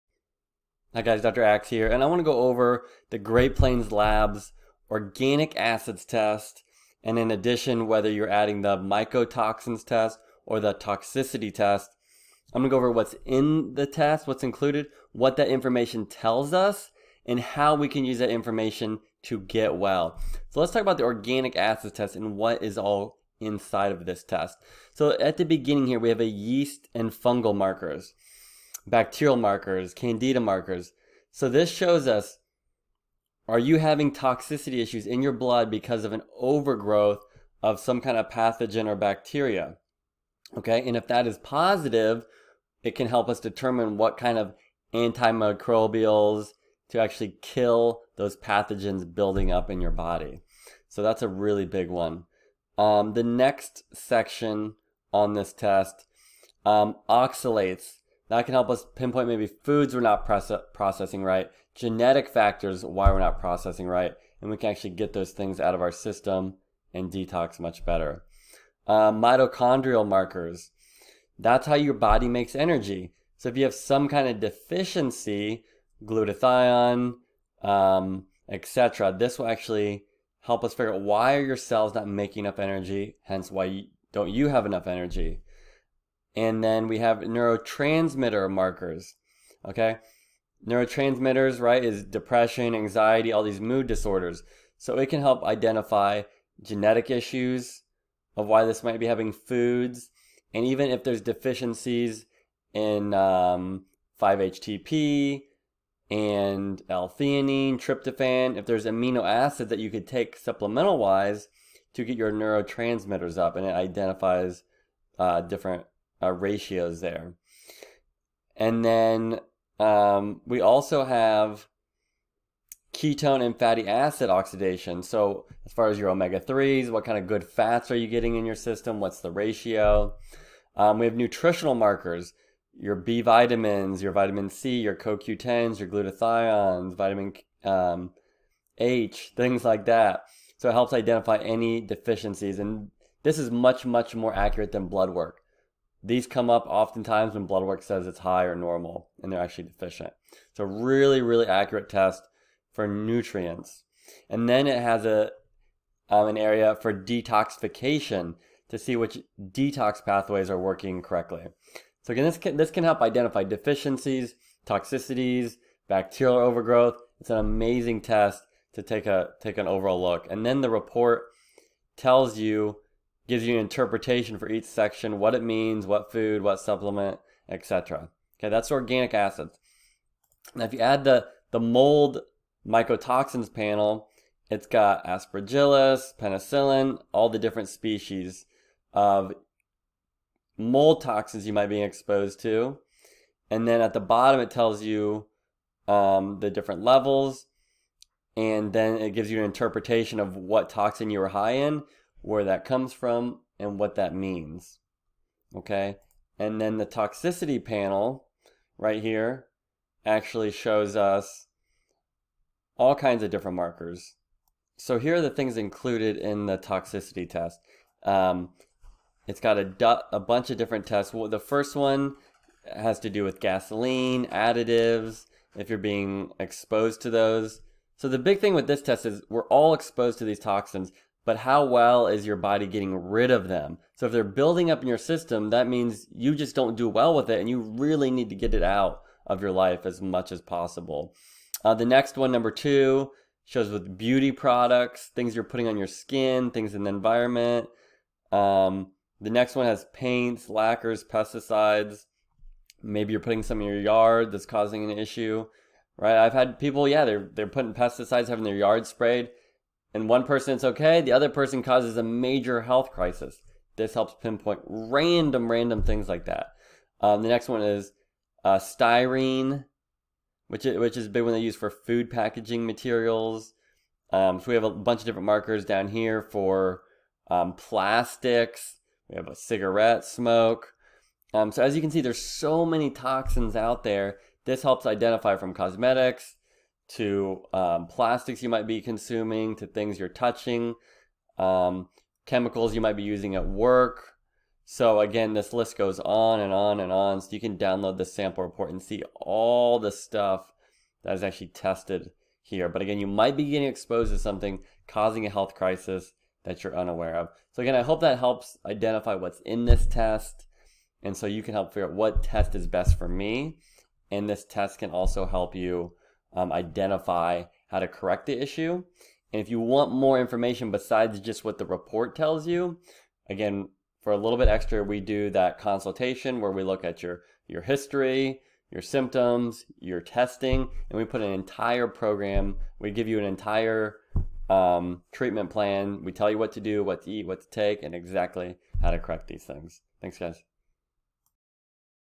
Organic Acid's & Toxicity Interpretation
Learn what's included in an organic acid's test, why it's more accurate than blood work, and how it can help you find your root cause!